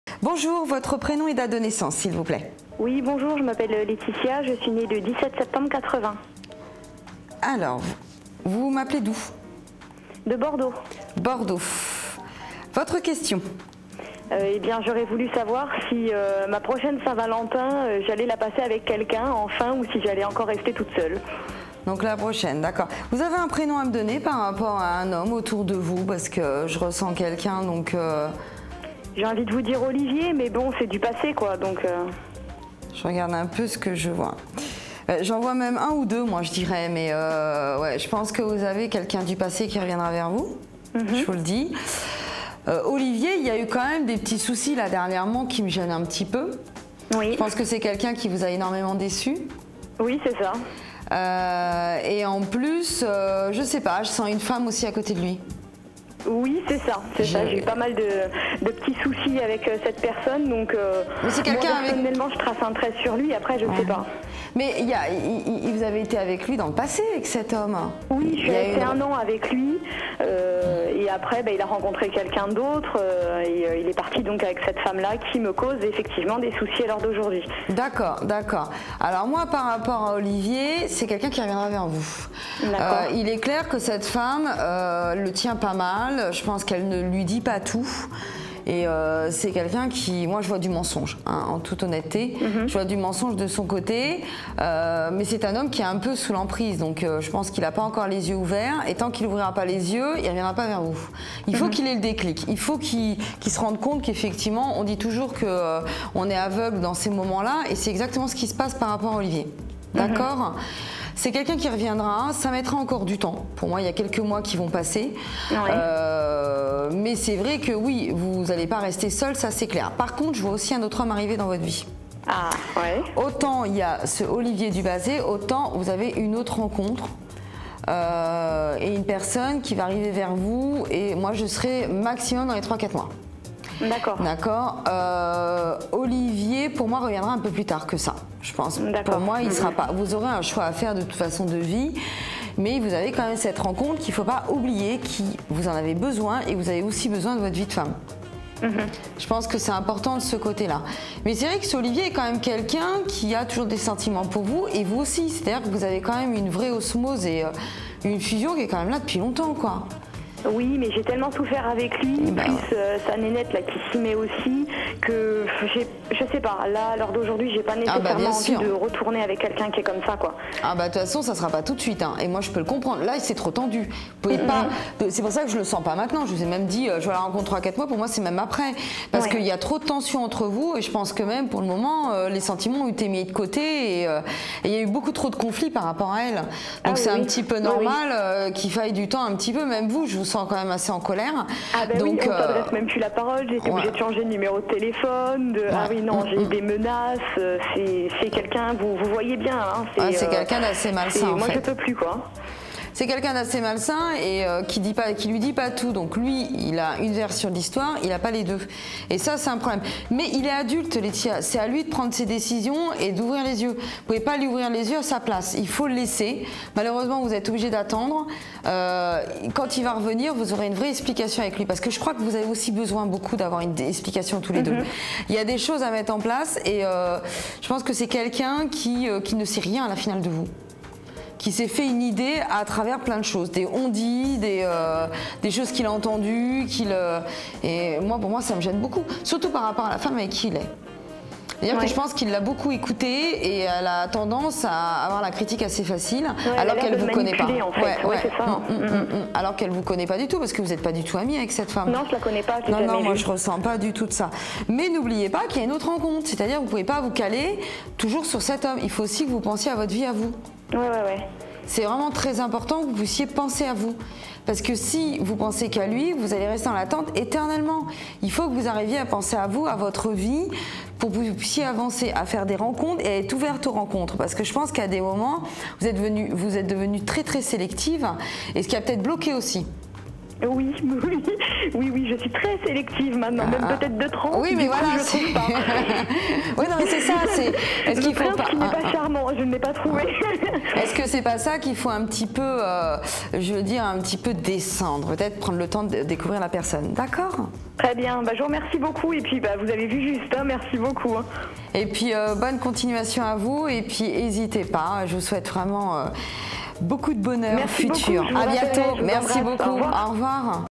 Quel est mon avenir sentimental? 100% Voyance - Peggy Yona
Peggy Yona répond à vos questions sur votre vie sentimentale.
Vous souhaitez faire une consultation avec Peggy Yona ? Rendez-vous sur sa fiche de présentation